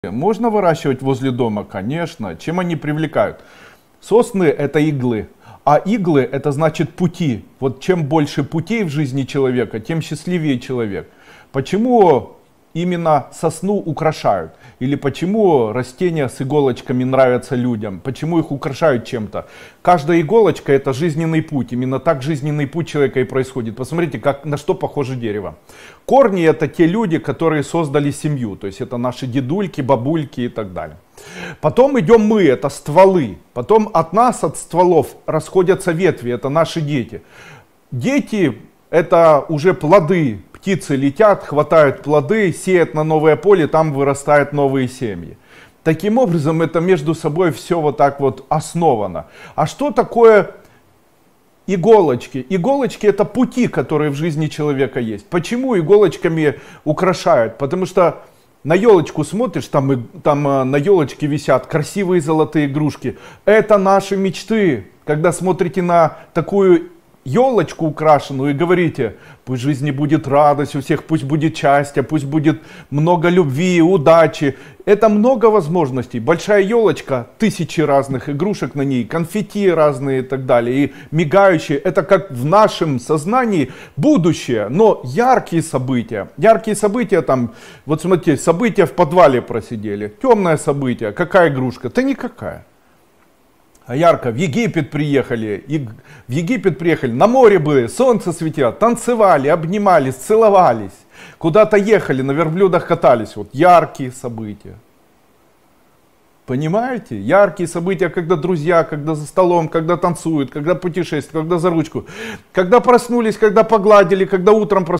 Символика елочек и сосен: Значение их роста рядом с домом и на праздниках
В этом видео мы обсудим интересный вопрос: можно ли выращивать сосны возле дома? Узнаем, почему сосны и другие хвойные деревья обладают особой символикой и как они могут стать направлениями в нашей жизни. Разберем, что означает традиция украшать елочку и какие значения могут нести плоды и иглы этих деревьев. Поговорим о корнях, стволах и плодах, как символических аспектах семейных отношений. Поделитесь своими мыслями в комментариях ниже.